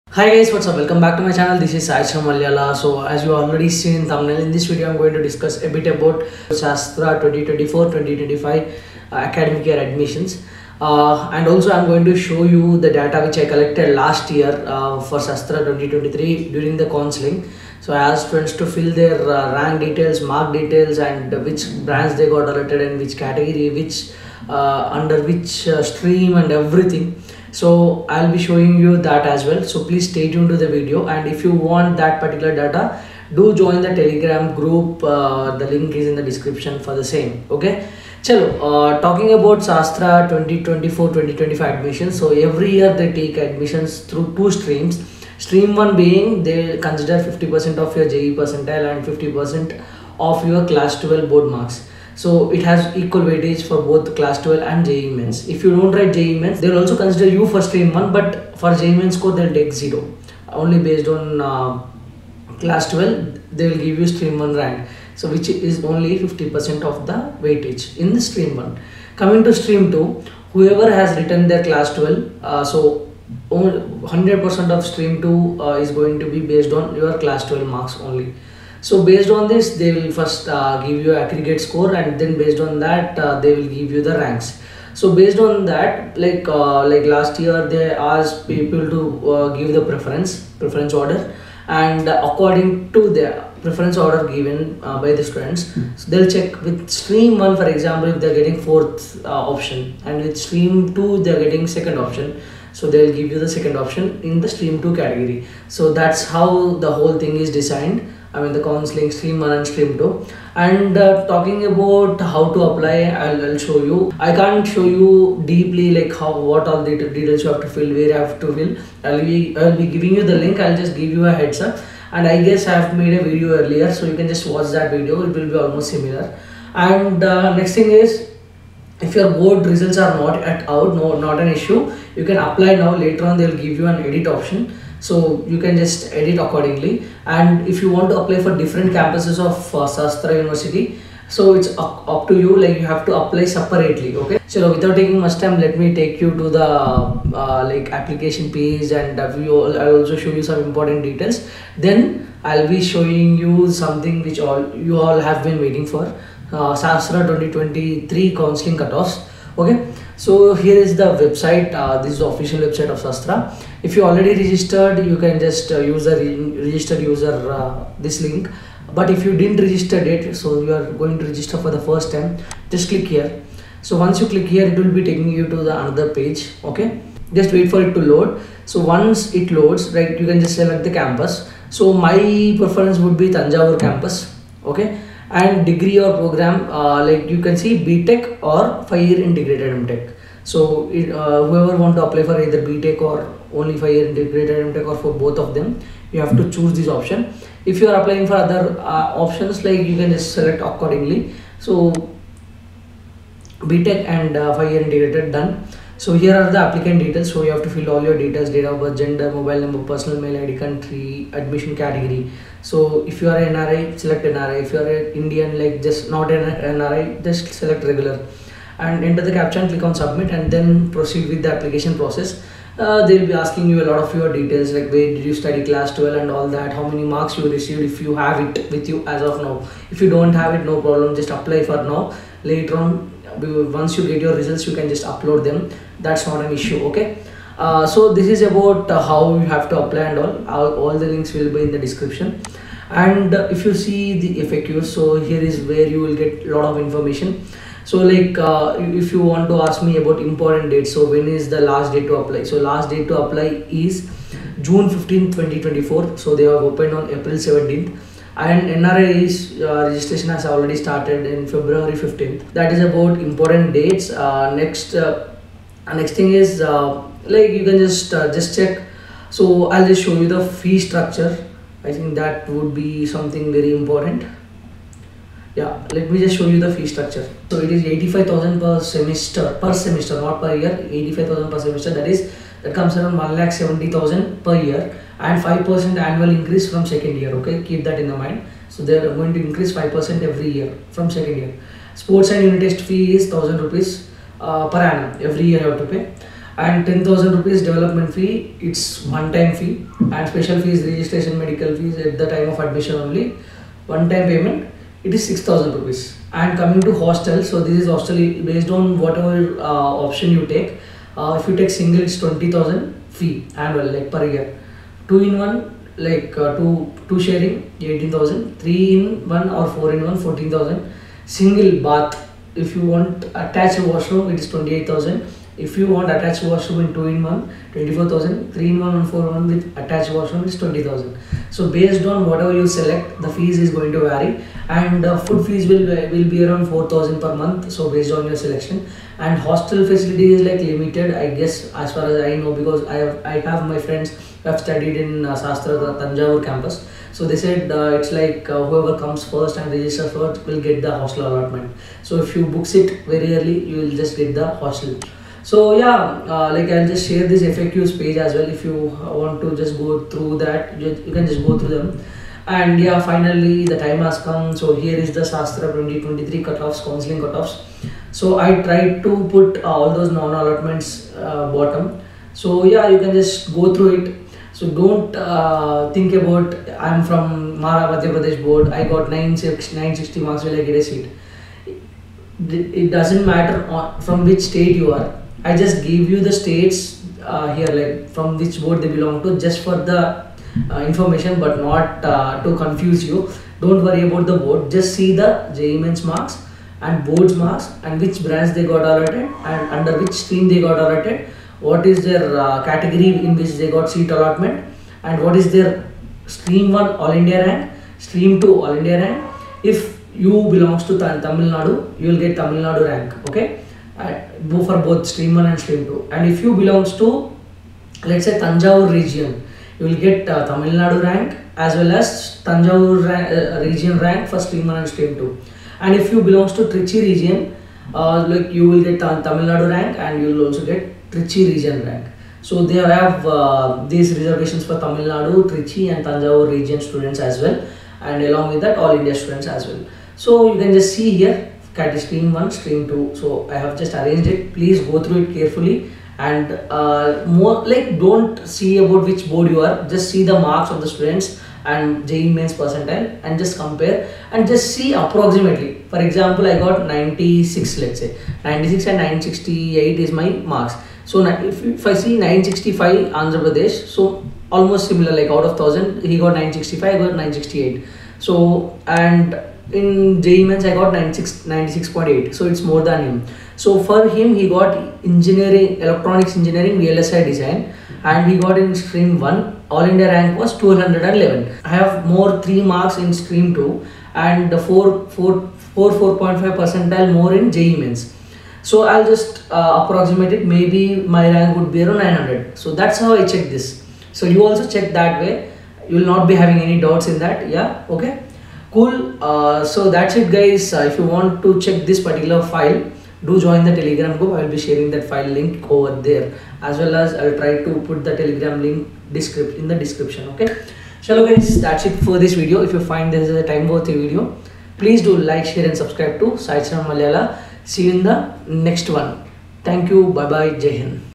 Sastra University Applications 2024 | Based on Class 12 and JEE | Cutoffs?
Call Me & Clear Your Doubts :
Any Queries:
Personal Details:
Sai Charan Malyala
B.Tech. Graduate